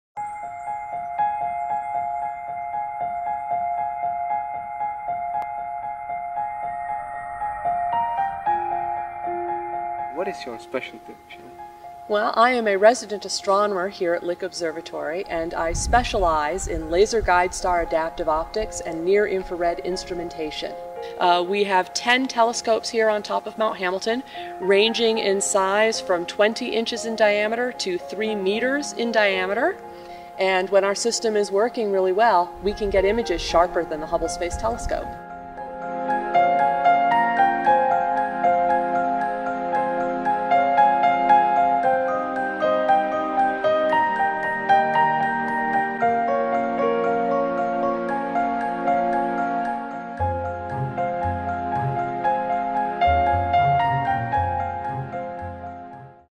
Elinor Gates - Looking deep into the universe, event invitation
Date: June 6th, 2013 at 7PM
Place: Era svět, Jungmanovo nám. 767, Prague
In this Creative Stream, we will connect with Elinor Gates from Lick observatory (California).

We will talk about topics like:
The search for extrasolar planets!
Laser guided adaptive optics!
Supernova explosions and discovery!